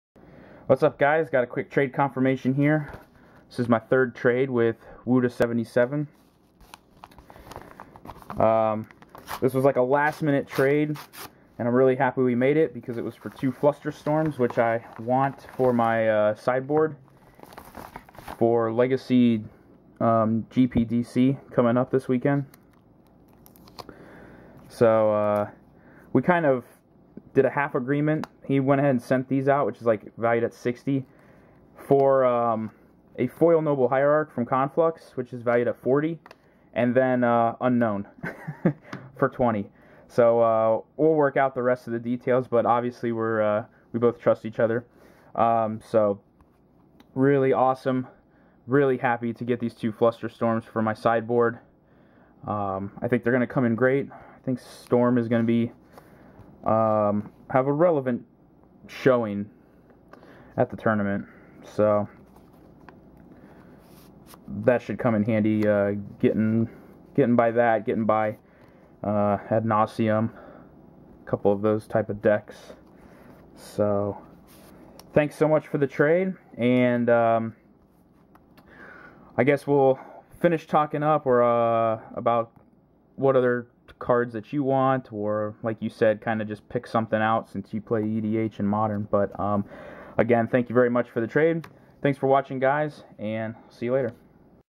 TC for Wuda77 #3 (x2 Flusterstorms!!!)
Great safe trader!  Thanks for hooking me up with x2 Flusterstorms for my Legacy Shardless BUG sideboard right before GP DC!